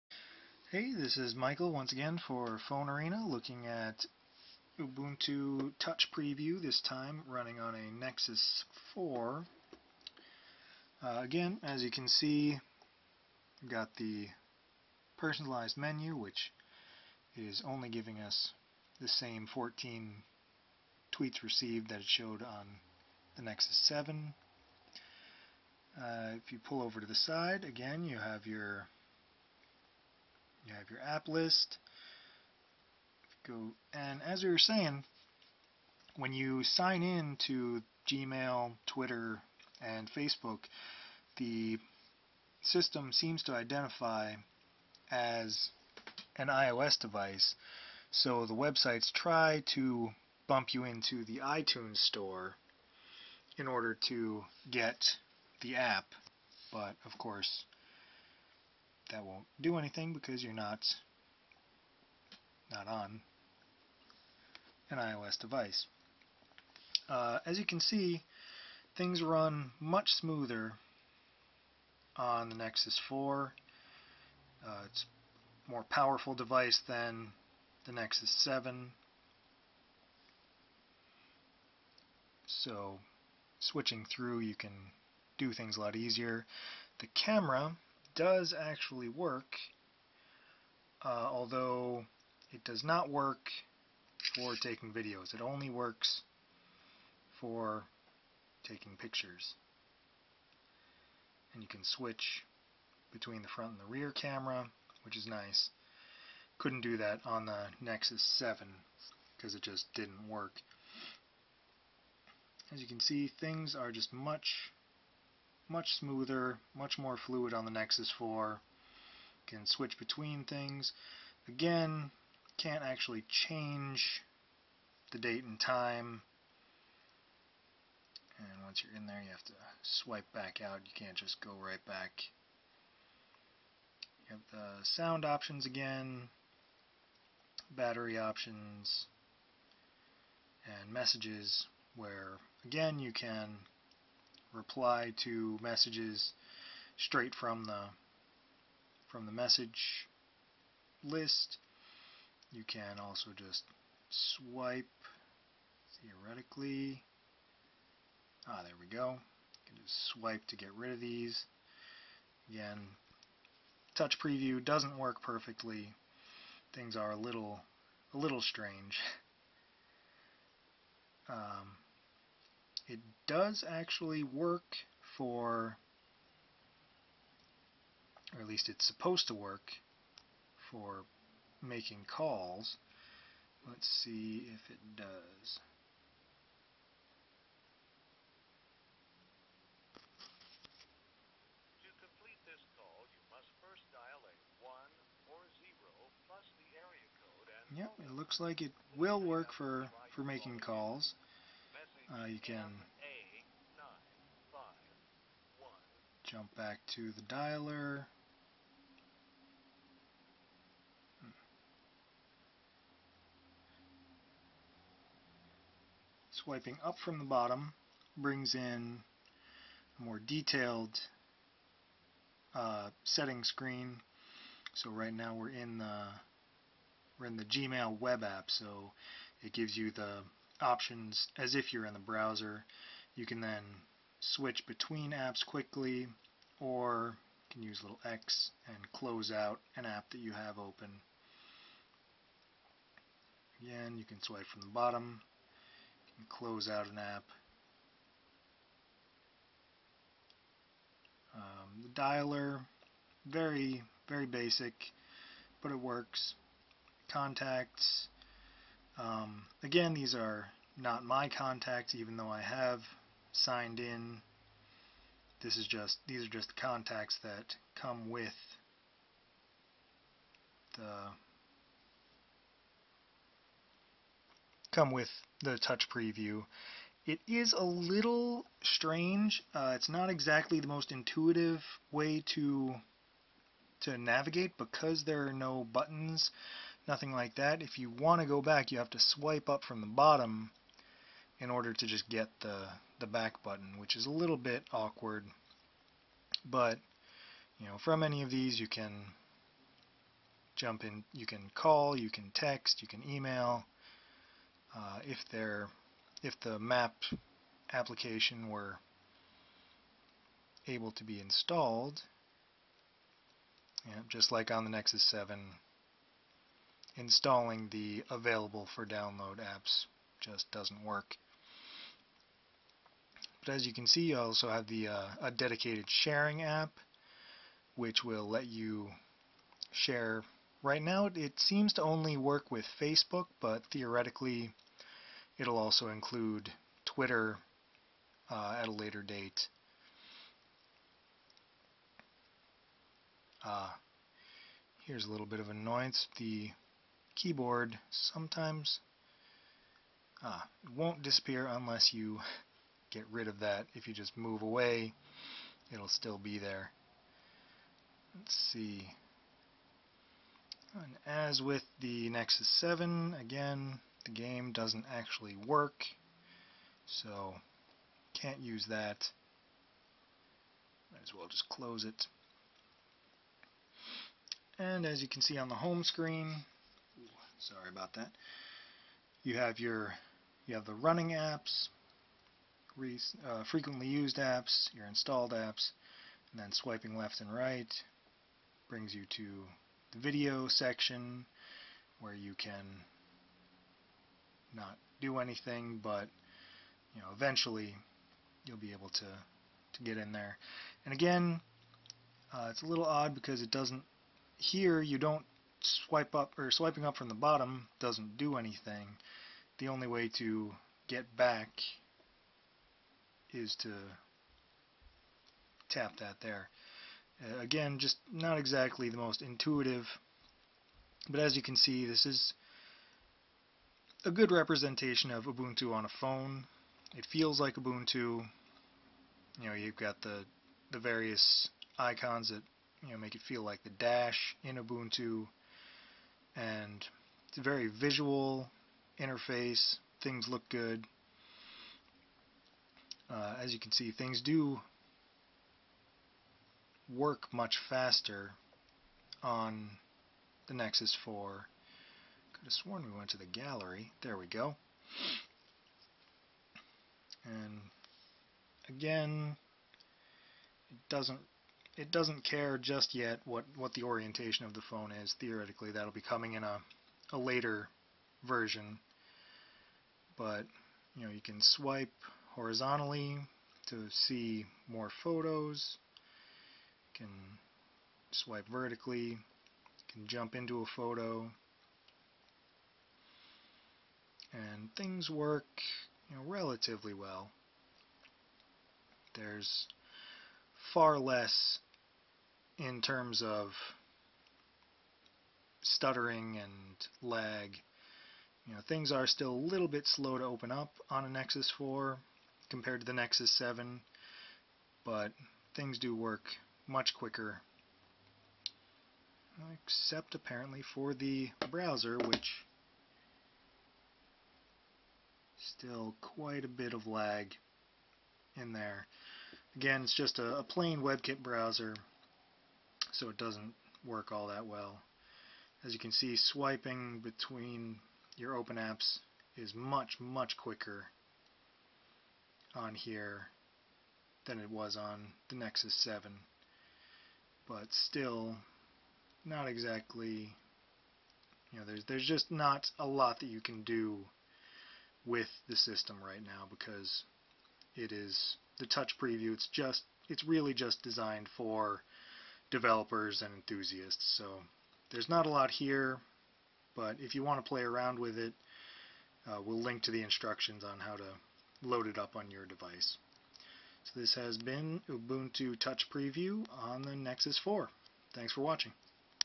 Ubuntu Touch preview for phones hands-on
Phone Arena presents a hands-on look of an early build of Ubuntu touch for phones.